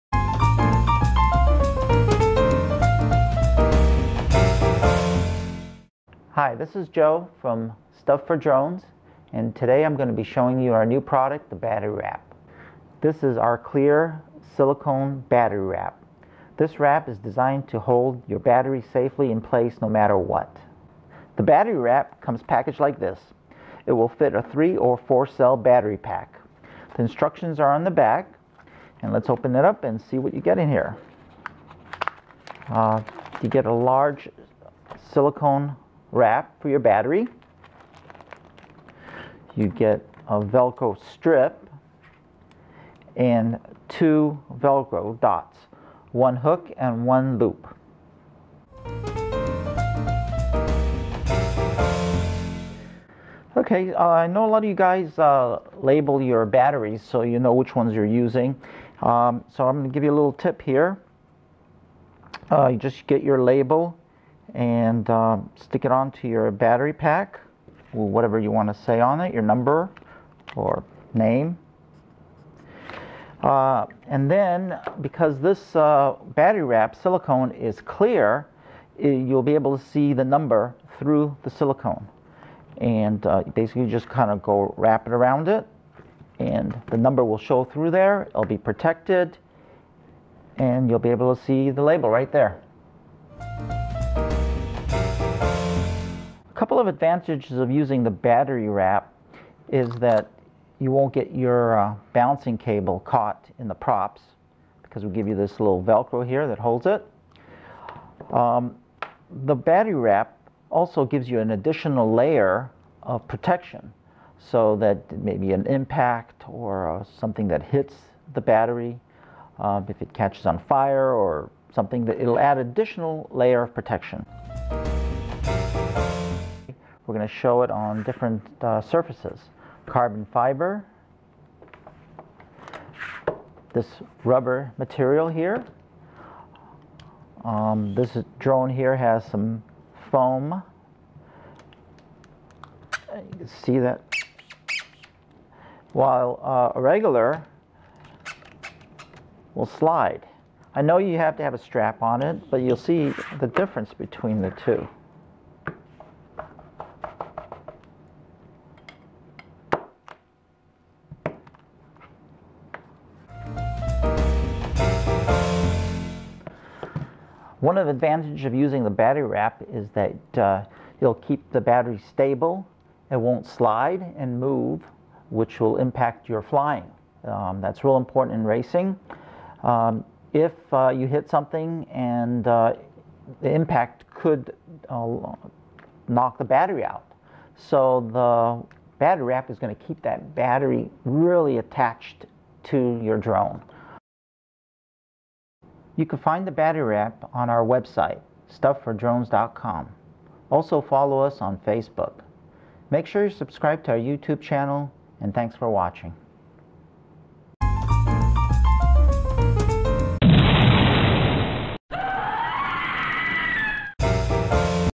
Shock Absorbing, Slip Resistant, Silicone Battery Wrap for Drones
Vibrations can cause your battery to slide slightly during flight and your drone can become unbalanced. Use this silicone battery wrap to keep your drone battery from moving. The adhesive wrap encases the battery and the silicone creates a slip resistant surface. The included Velcro (TM) anchors your balancing connector, keeping it out of the way of the props.

Fits standard 3 and 4 cell RC Batteries.